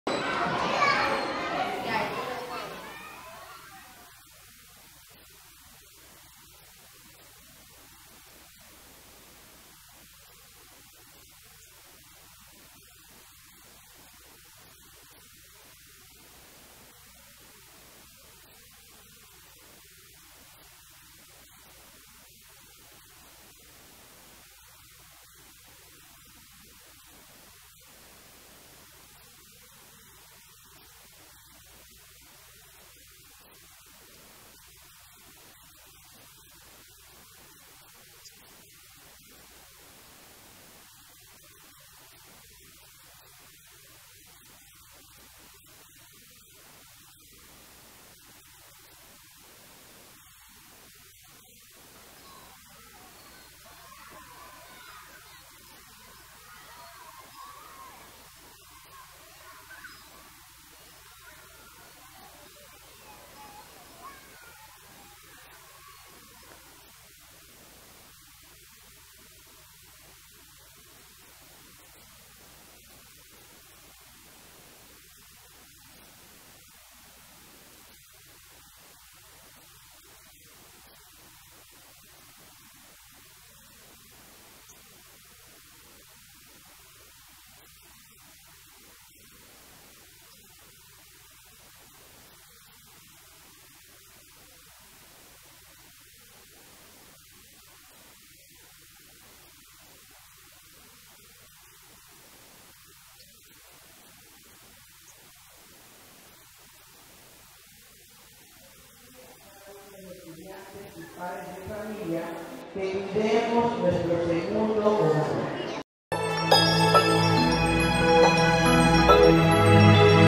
25 años de solidaridad a través de la escuela Juan Martín Moÿe
Con la ayuda y solidaridad de personas e instituciones, la escuela Juan Martín Moÿe, ubicada en la isla Trinitaria, ha logrado ofrecer educación particular de calidad por 25 años con una pensión asequible a padres de escasos recursos.